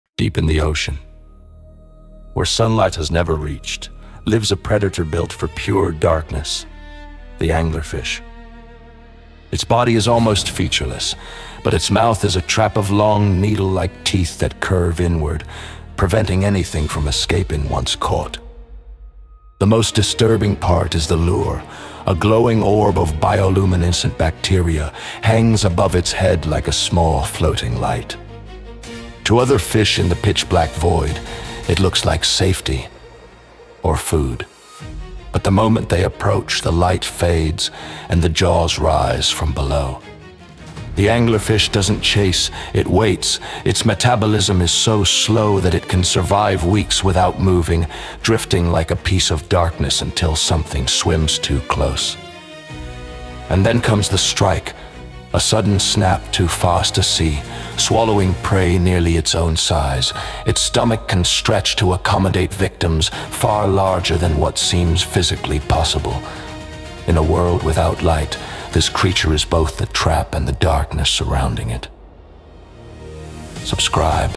I Found The Weirdest Fish In The Ocean
The anglerfish is one of the deep ocean’s most terrifying hunters.
In complete darkness, it uses a glowing lure to draw prey straight into its jaws.
This strange creature survives where almost nothing else can — under crushing pressure, freezing temperatures, and eternal night.
Watch how this deep-sea predator turns the darkness into its greatest weapon.